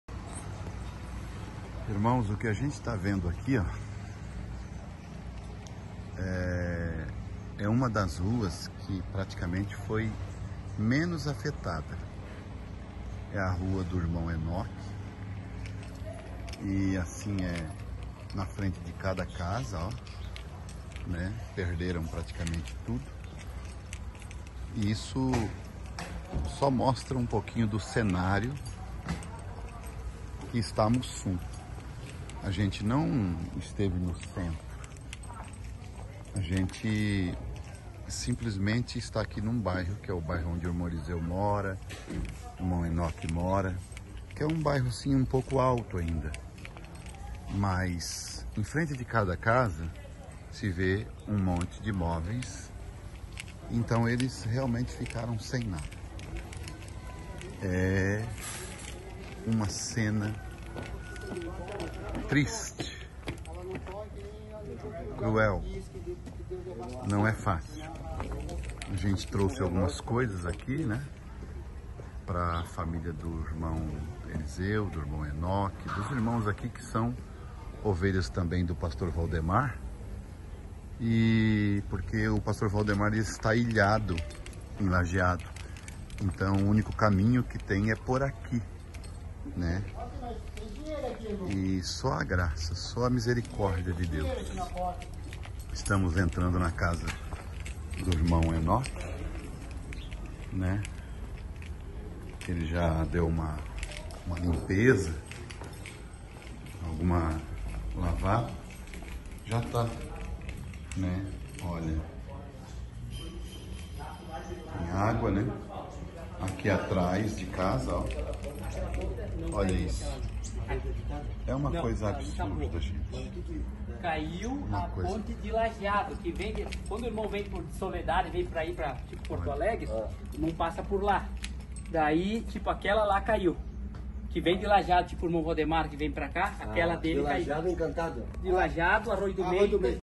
CENAS CRUÉIS
cidades inteiras foram destruídas pela enchente no sul do Brasil...